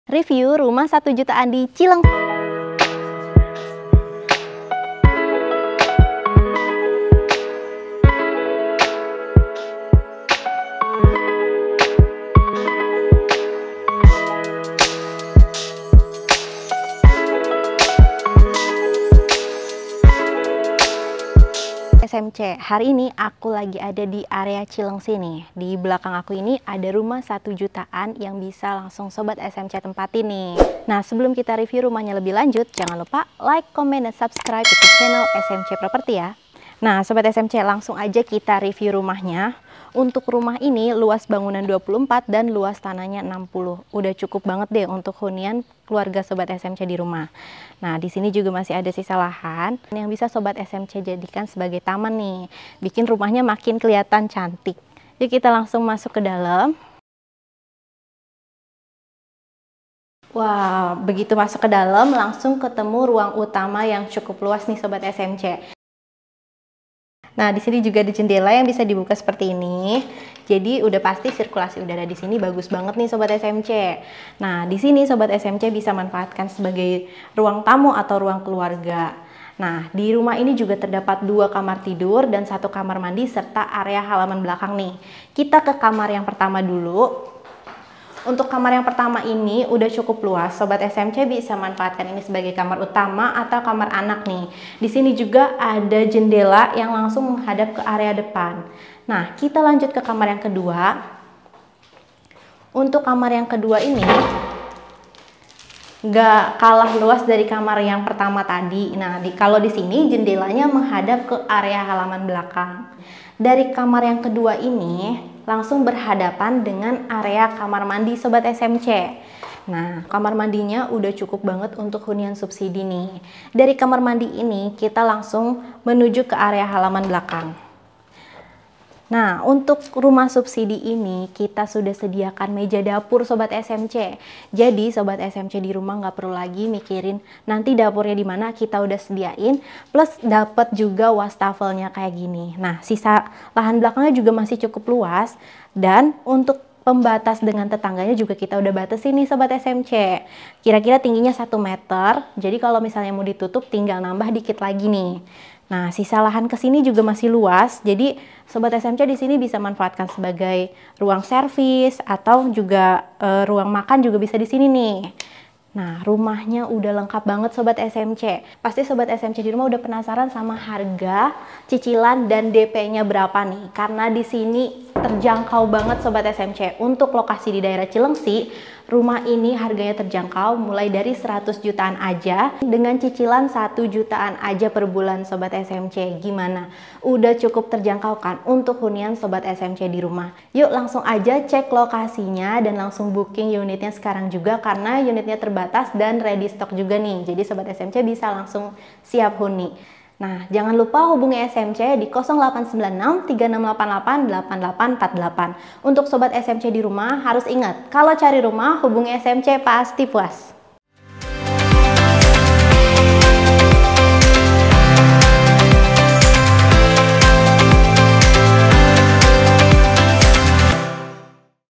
BURUAN BOOKING UNITNYA ! LAGI ADA PROMO DP 2 JUTA SUDAH FREE BIAYA-BIAYA !!CILEUNGSI | SMC PROEPRTY
Lagi promo CUKUP DP 2 JUTA aja sudah ALL IN ‼
Yuk segera booking dan jangan sampai kehabisan unitnya!

✅ Cicilan mulai 1 Jutaan/bulan
✅ LB 24 / LT 60
✅ Free Biaya-biaya‼

Langsung survei dan booking unitnya sekarang ya 😊✨